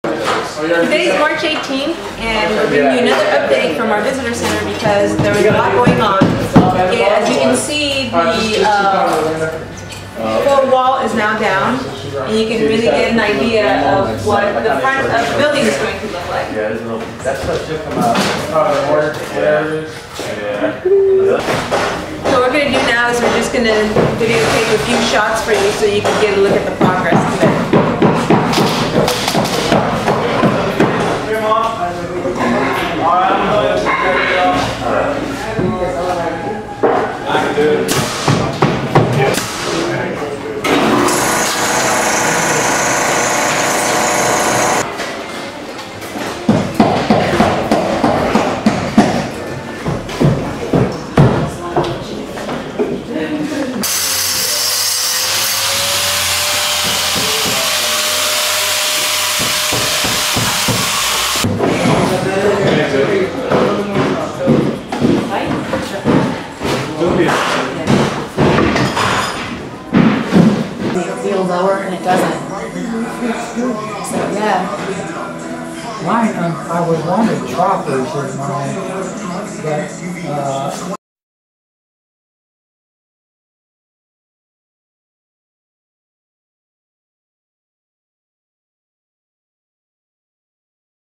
Visitor Center Update: March 18, 2009
It's only been two days since our last update and there are already significant changes to the building!  Villa Finale is the first National Trust Historic Site in the state of Texas.  To find out more about the site and our work, please